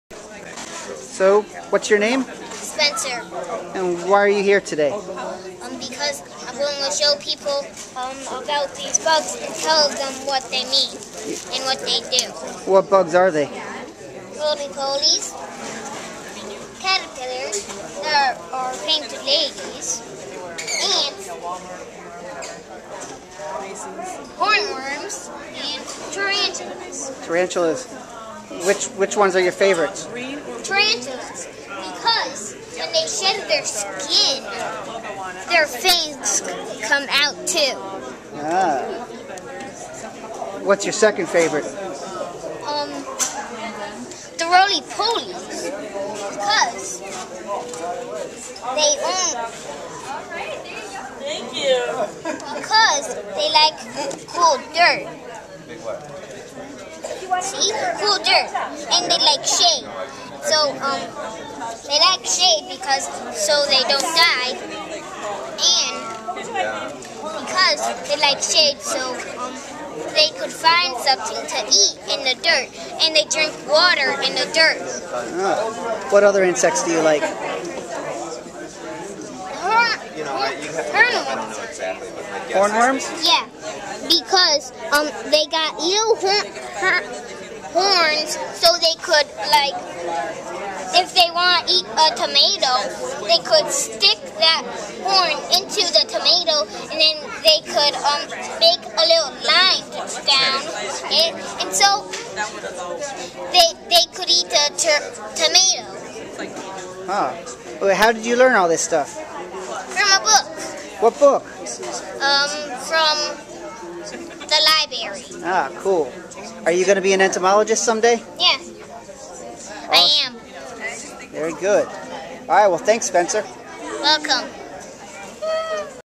A Future Entomologist Talks About Insects
An interview with Spencer, a first-grader from Mary Hooker Env Science Magnet School, at the "It's a Bug's Life" Insect Fair held at the 2012 ESA Eastern Branch meeting in Hartford, CT, March 2012.